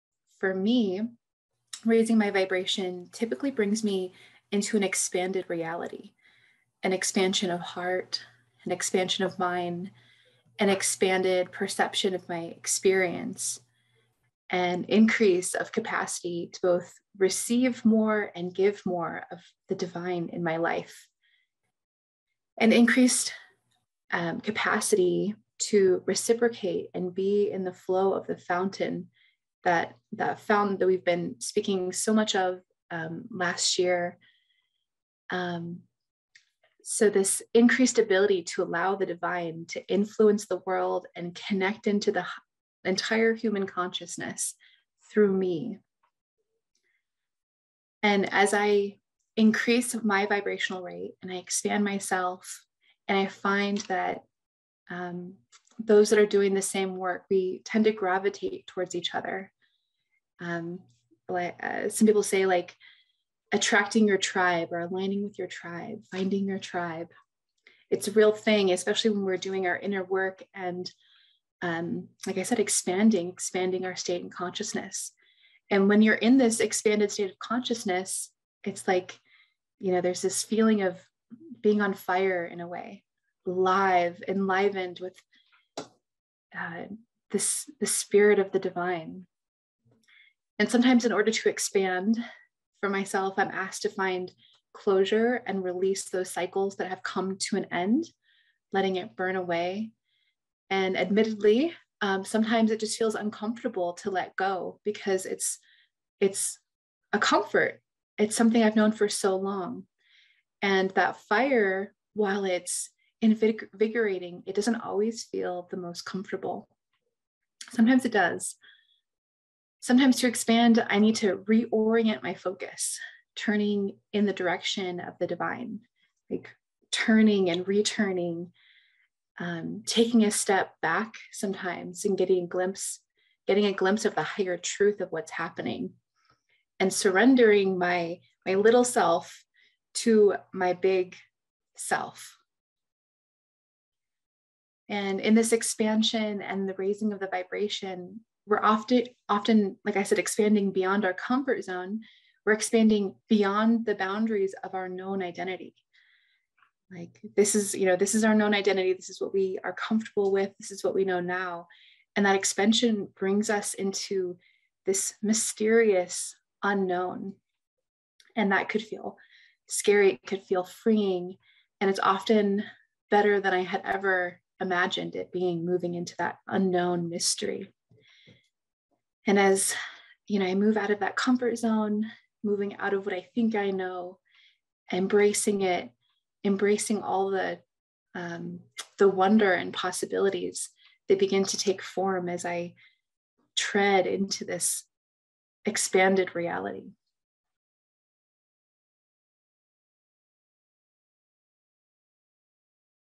Lana Roach: An Expanded Reality
Reiki Master Teacher Lana Roach: "An Expanded Reality" excerpt from Sunday service on January 23rd, 2022 at Sunrise Ranch in Loveland, Colorado.